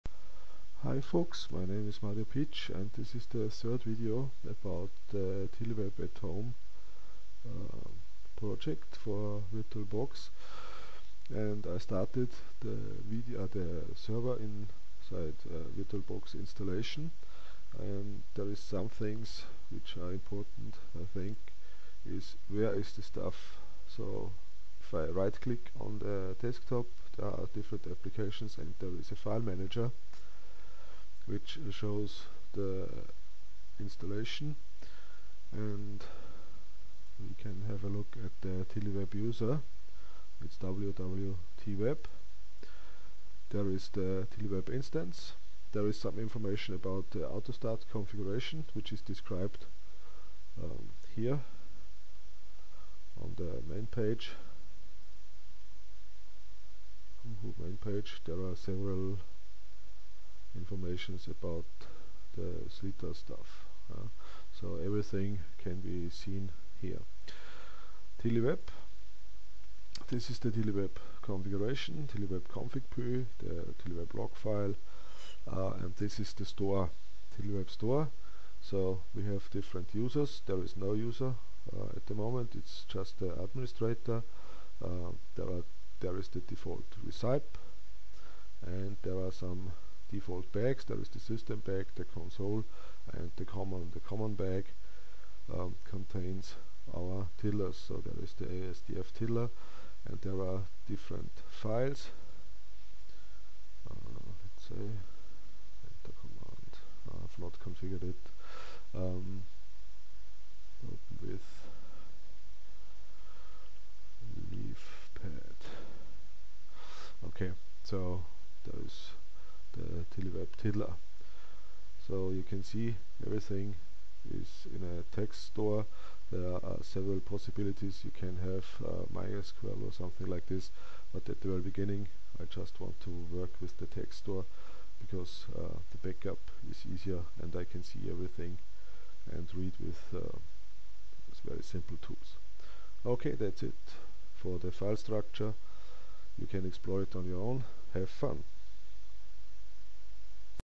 TiddlyWeb at Home Directory Structure
A short overview about the TiddlyWeb instance directory structure.